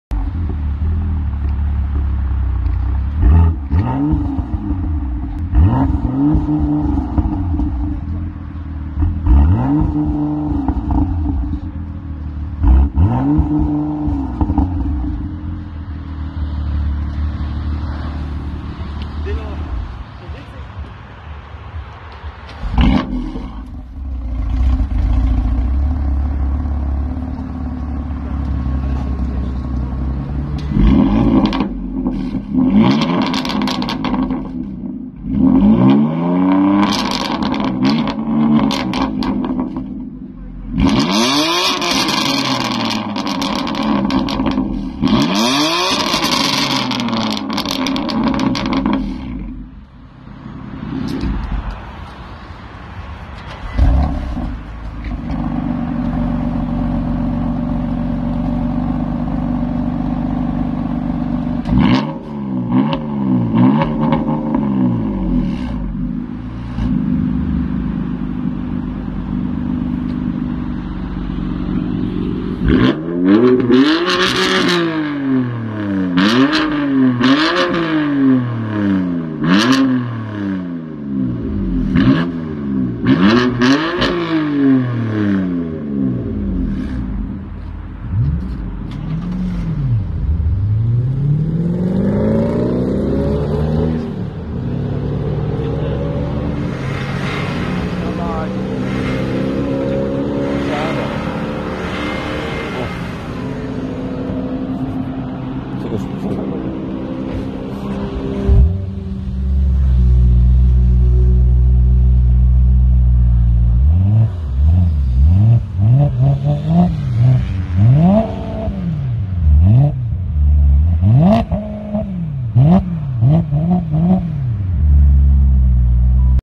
EXHAUST SOUND WAR BMW F87 M2 500BHP vs M4 700BHP vs M4 600BHP vs E92 M3 V8 450BHP vs F30 335D 450BHP
Bmw battle exhaust sounds loud.

Straight pipes bmw m4 m2 m3 V8 335d f30 wirh hybrid turbo !!!

Bmw wydech glos rury przelot ale napierdala po uszach !!!